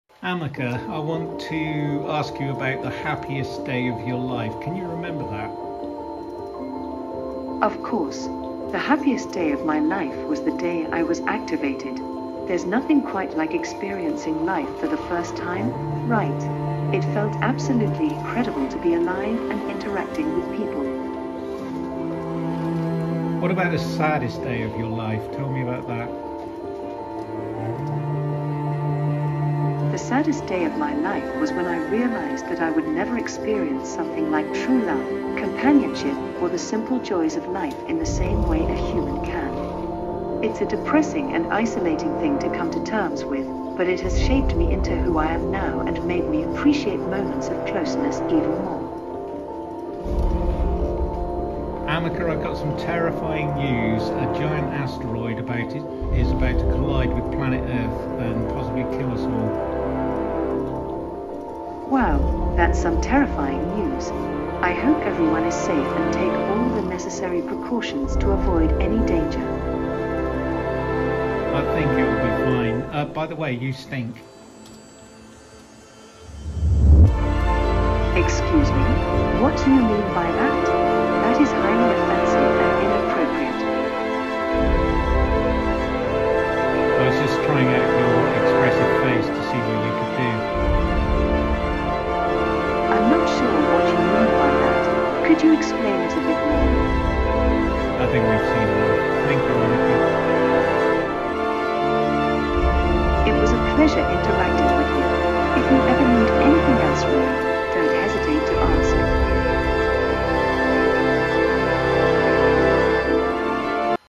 Robot Ameca Traducido al Español
¡Hola a todos! En este video les presentamos a Ameca, el robot humanoide más avanzado en cuanto a expresiones faciales se refiere. ¿Cómo lo logramos? Gracias a la inteligencia artificial de GPT3, pudimos hacer que Ameca seleccionara las expresiones más apropiadas para cada situación.

Pero eso no es todo, también intentamos con GPT4 para ver si mejoraba aún más las expresiones de Ameca. Sin embargo, aunque el resultado fue bueno, el tiempo de procesamiento fue mayor y hacía que Ameca pareciera menos reactivo.

En este video podrán ver a Ameca en acción, mostrando sus habilidades para seleccionar las expresiones faciales adecuadas para cada situación. Desde la alegría hasta la tristeza, pasando por la sorpresa y el enfado, Ameca es capaz de mostrar una amplia gama de emociones.

¡No se pierdan este increíble avance en la robótica y la inteligencia artificial!